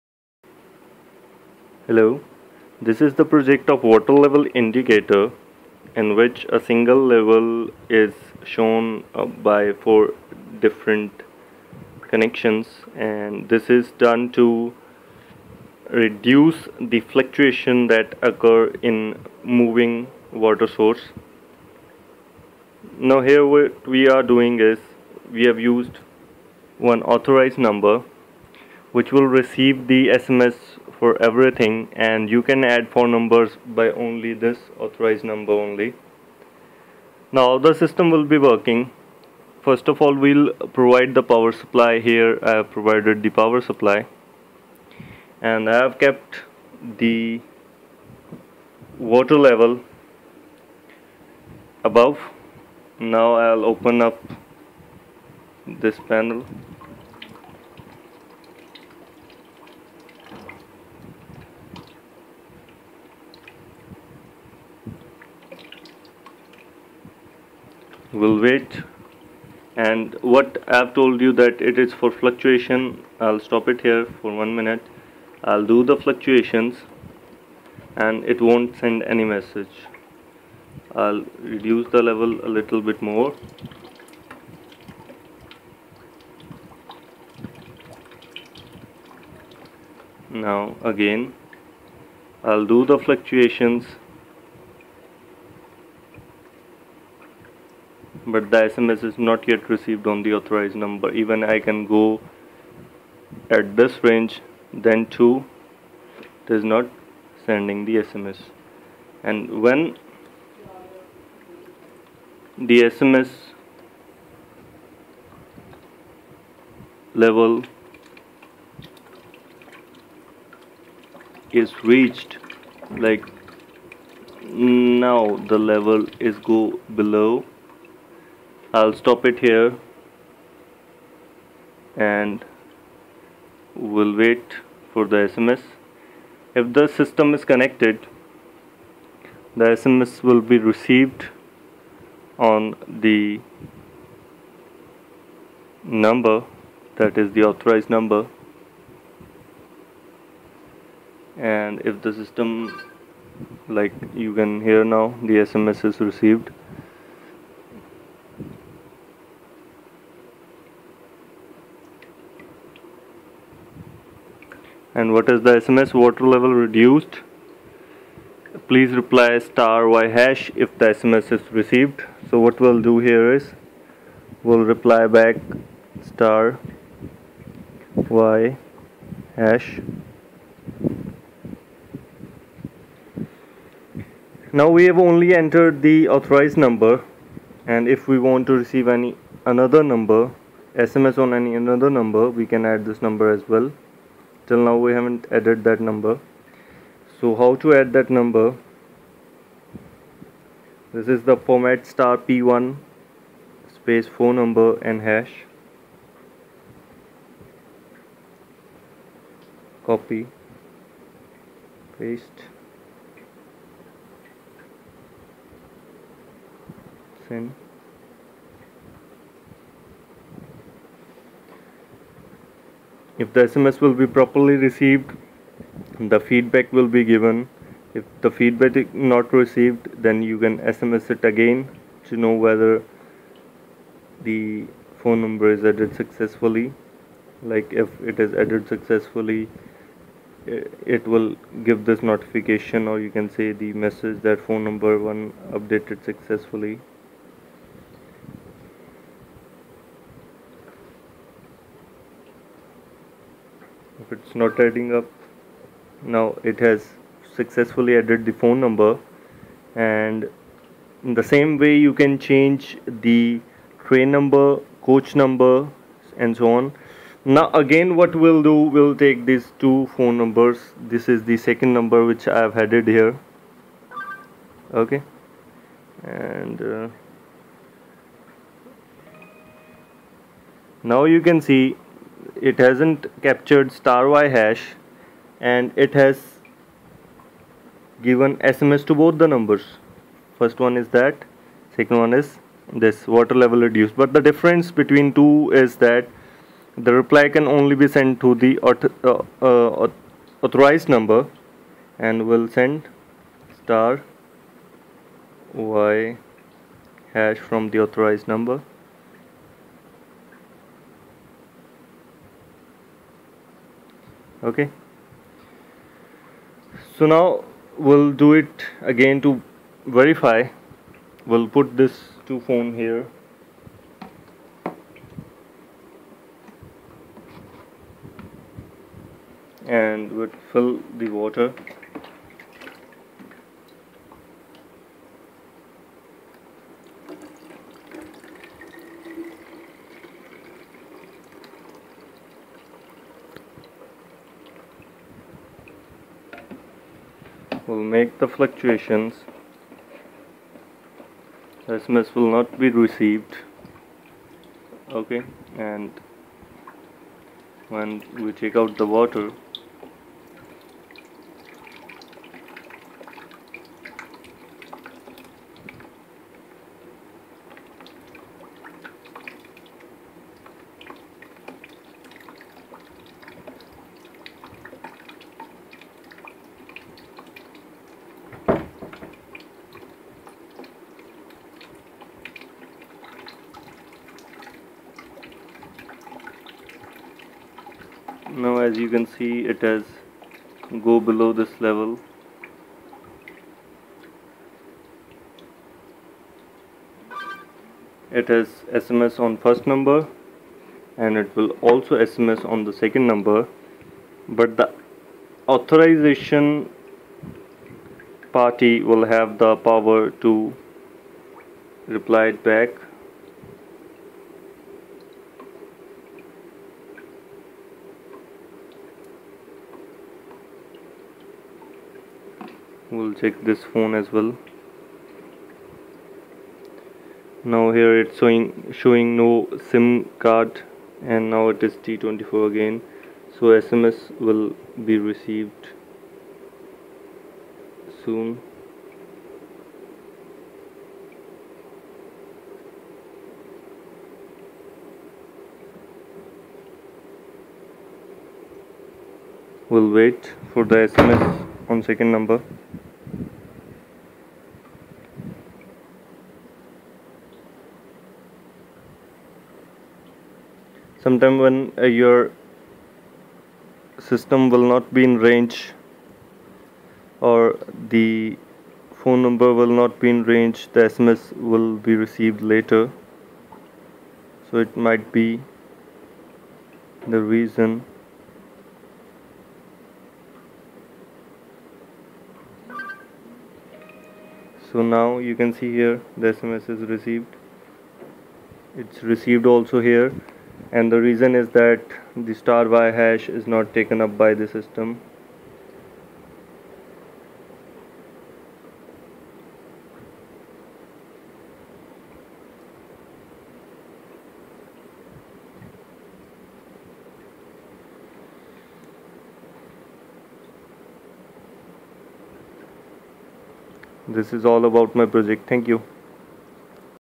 Water Level indicator for movable units like railways via SMS
As the water of railway tank got reduced so this system will SMS on authorized number to give indication